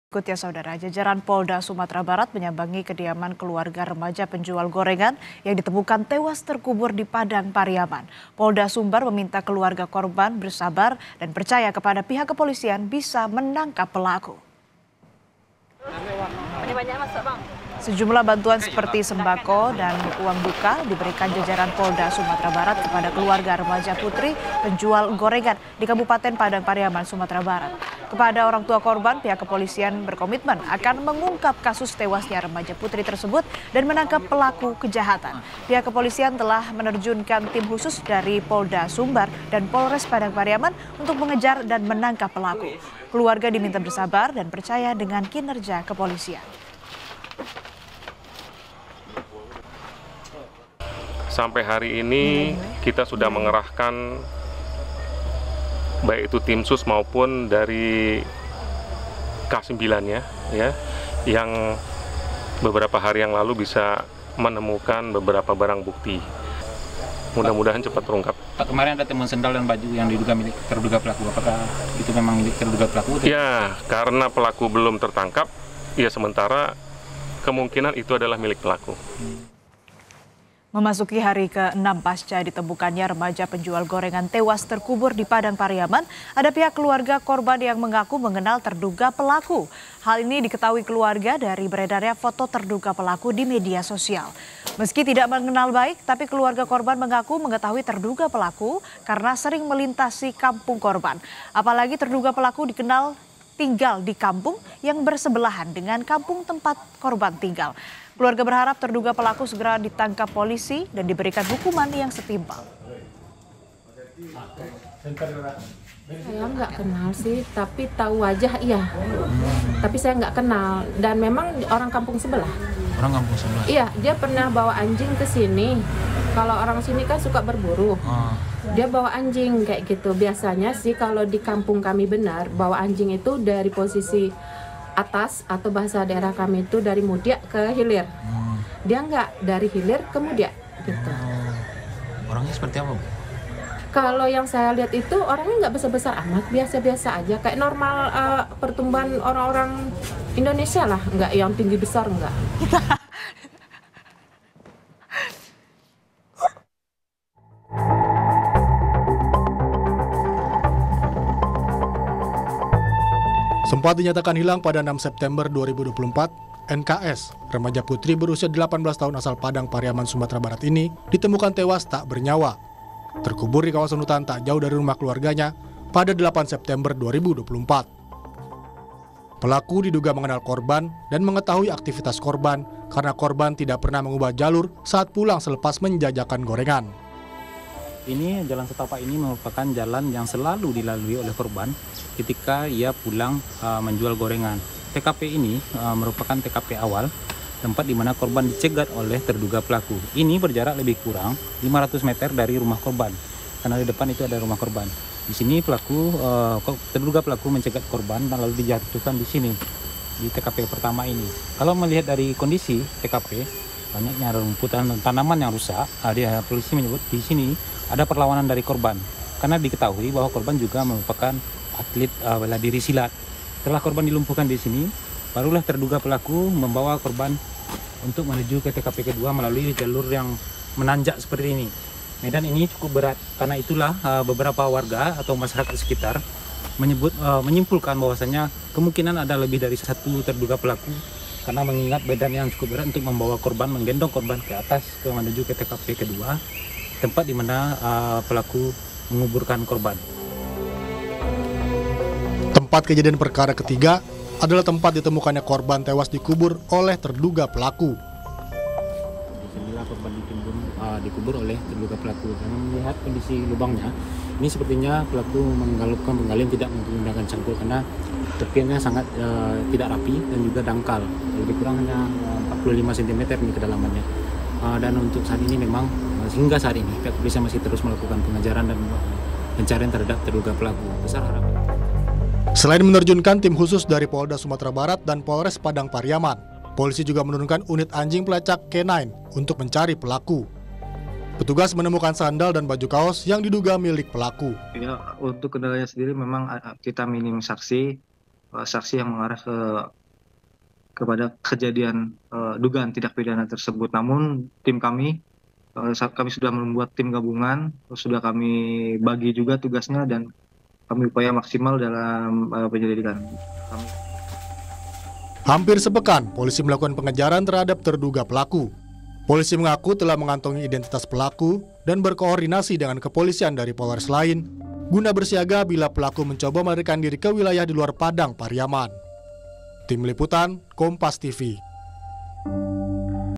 Keluarga Korban Pembunuhan Gadis Penjual Gorengan Mengenal Diduga Pelaku
KOMPAS.TV - Terduga Pembunuh Gadis Penjual Gorengan Dikenal Warga.

Untuk lebih tahu berita terupdate seputar Jawa Barat, bisa klink link di bawah: